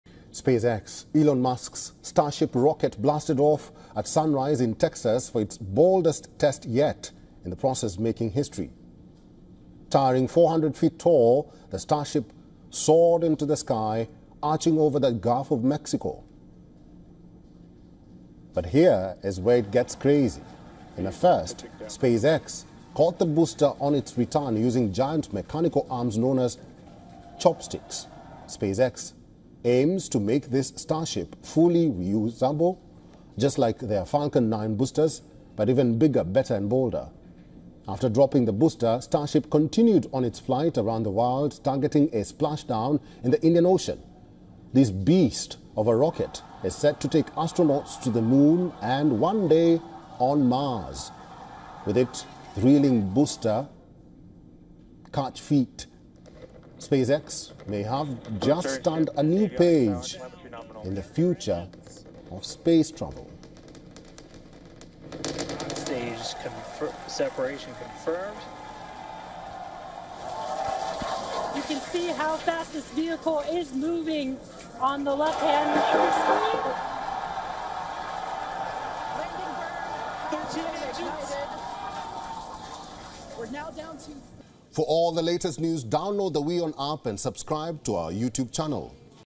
Spacex Starship Launch: Booster Catch Steals the Show | World News | WION
SpaceX Elon musk's Starship rocket blasted off at sunrise in Texas for its boldest test yet in the process making history. Watch to know more.